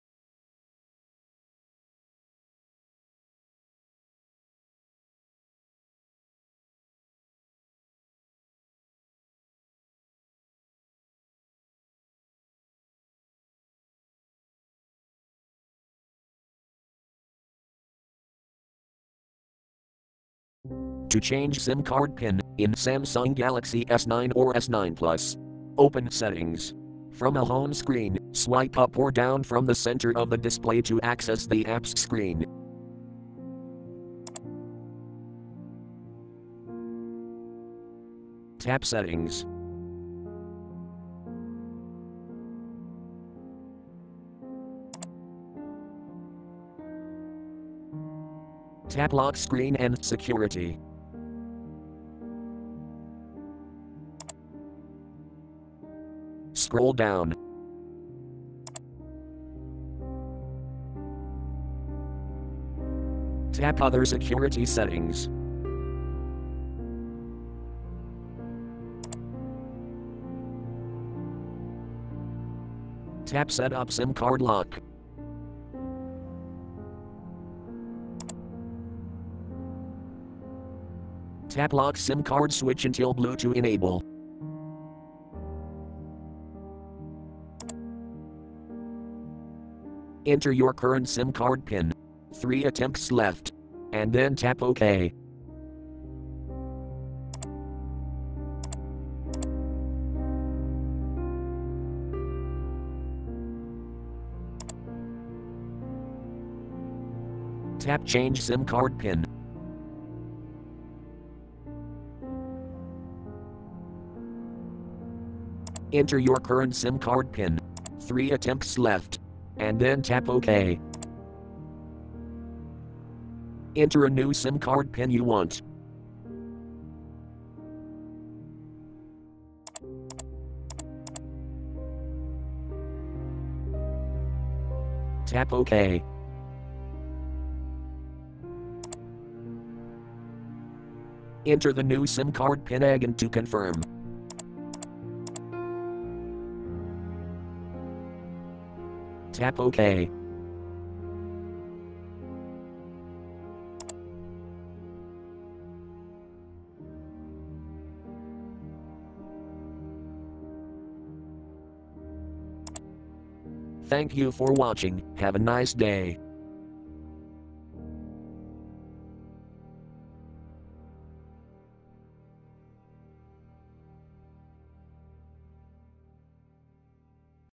Samsung Galaxy S9 : How to Change SIM card PIN (Android Oreo)
This video show How to Change SIM card PIN in Samsung Galaxy S9 or S9+.  In this tutorial I use Samsung Galaxy S9 SM-G960FD Duos International version with Android 8.0 (Oreo).